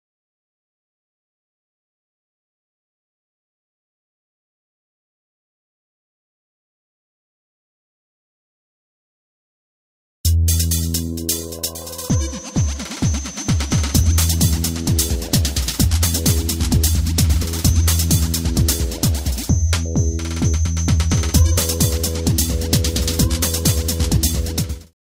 [as]-style bump - TV Anim. Scheduling Represented with the Characters - 2019 Redux [4K]
Based on the 'TV Animation Scheduling Represented with the Characters' bumper from October 26, 2017.